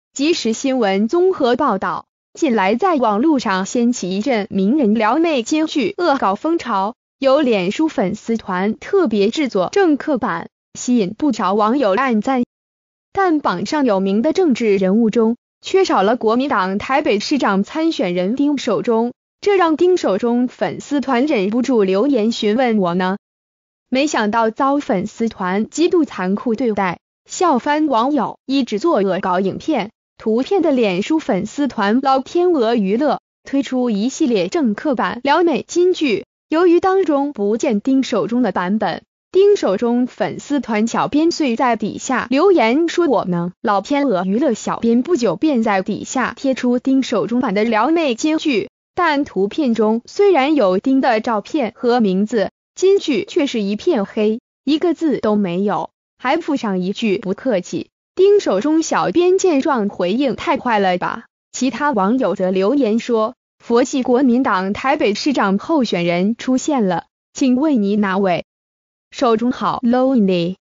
撩妹金句正夯！丁守中登門求圖 小編回應超傷人...
但榜上有名的政治人物中，缺少了國民黨台北市長參選人丁守中，這讓丁守中粉絲團忍不住留言詢問「我呢！」沒想到遭粉絲團極度殘酷對待，笑翻網友。

發表評論評論如下。